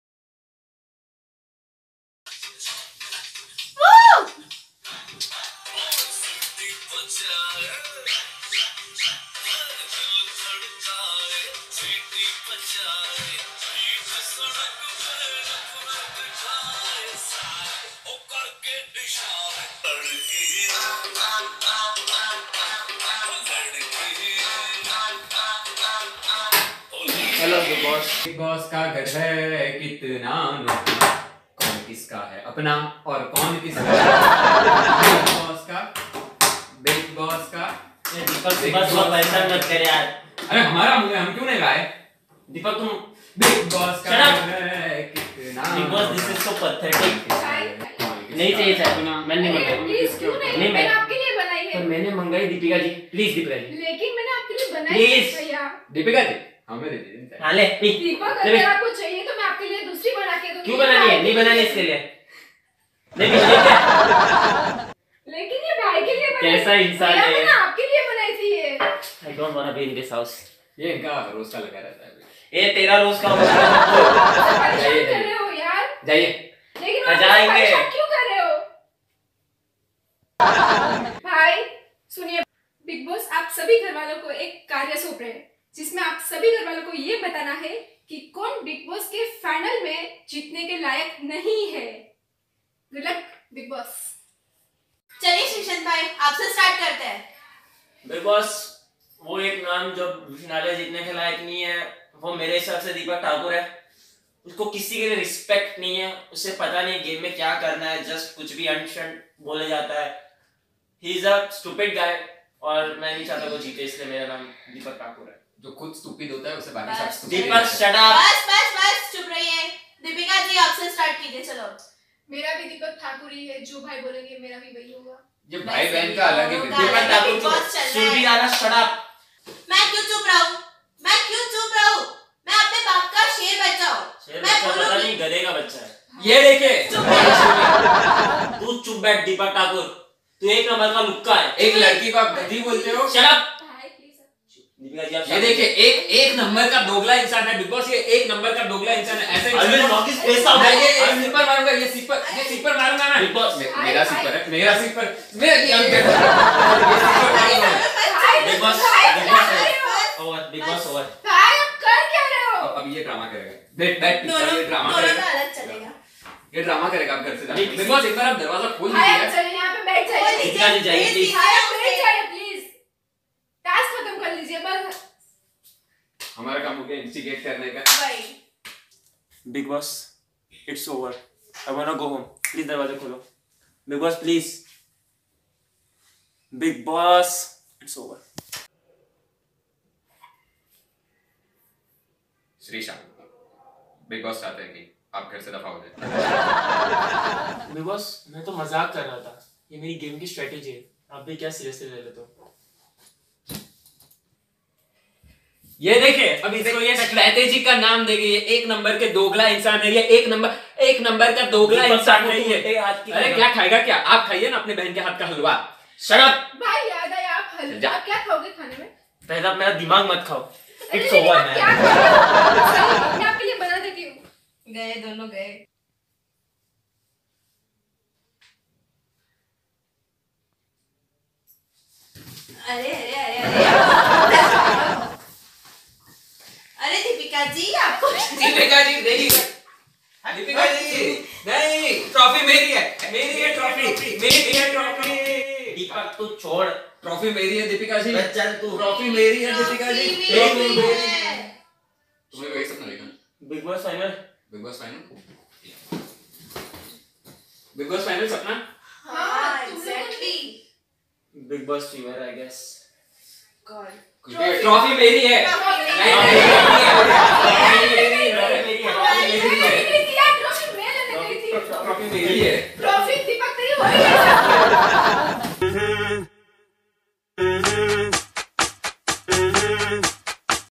Winner of Bigg Boss 12 LEAKED ? | Funny Spoof | Viral Swag TV
Winner of BB12 leaked?
do checkout this video to know the winner of BB12.

Video Credits:-

Actors according to their Characters;
Surbhi Rana:- Dhara Hajare
Deepak Thakur:- Vivek Surve
Dipika Kakkar:- Chandni Sonaiya
Sreesanth:- Pritesh Mishra

Editor:-
Rutvik Barot

Produced by:-
UNIQUE WORLD PRODUCTIONS

If you liked the Video than,

Viral Swag TV
by UNIQUE WORLD PRODUCTIONS